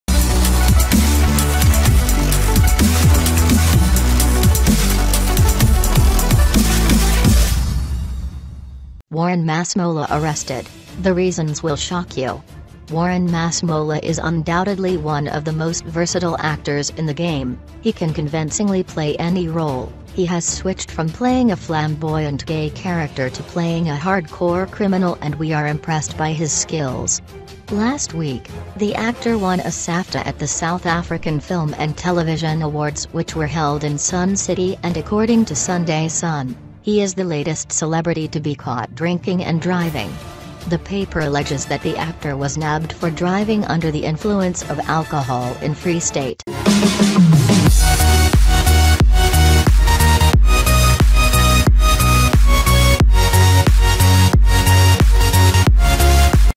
Warren Masemola arrested, the reasons will shock you
Warren Masemola arrested, the reasons will shock you - Warren Masemola is undoubtedly one of the most versatile actors in the game, he can convincingly play any role, he has switched from playing a flamboyant gay character to playing a hardcore criminal and we are impressed by his skills. Last week, the actor won a SAFTA at the South African Film & Television Awards which were held in Sun City and according to Sunday Sun, he is the latest celebrity to be caught drinking and driving. The paper alleges that the actor was nabbed for driving under the influence of alcohol in Free State.